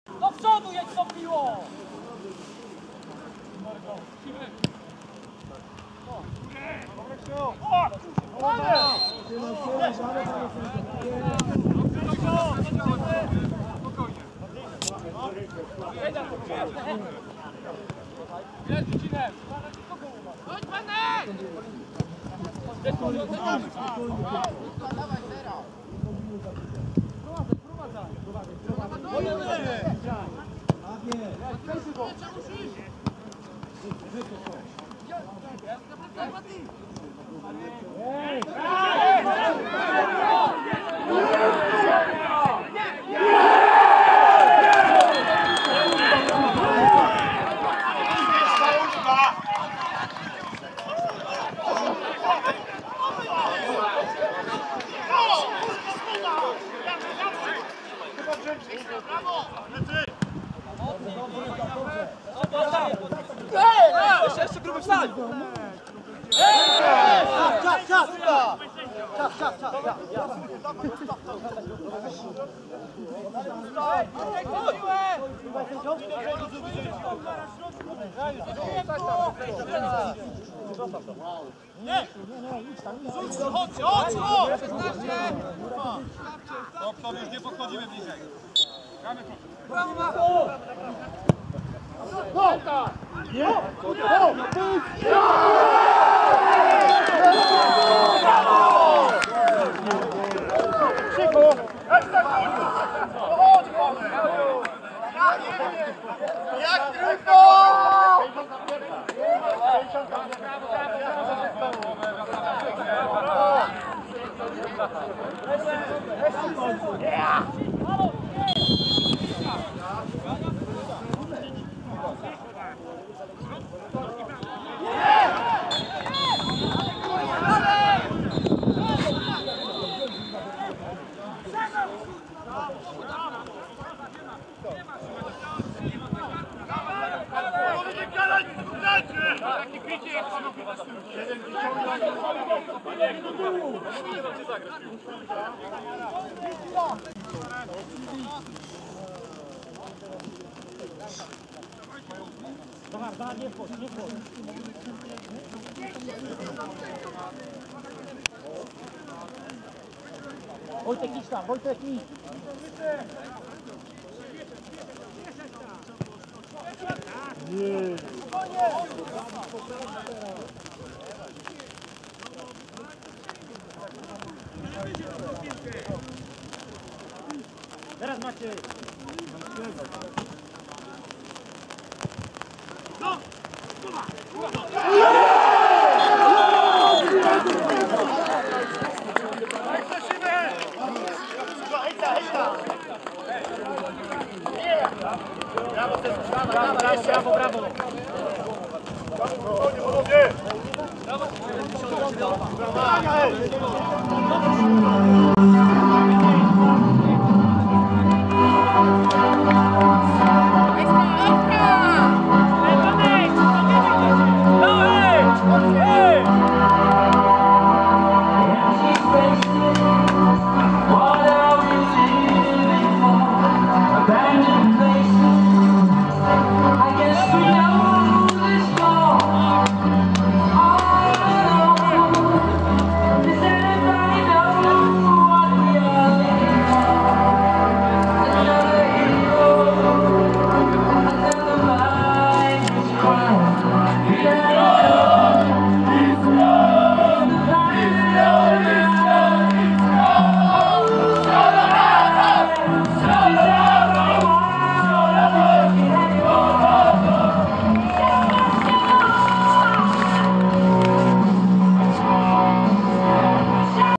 Iskra Szydłowo 3:1 Czarni Wróblewo
Bramki z meczu IV ligi wielkopolskiej północ, rozegranego dnia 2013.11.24 w Szydłowie.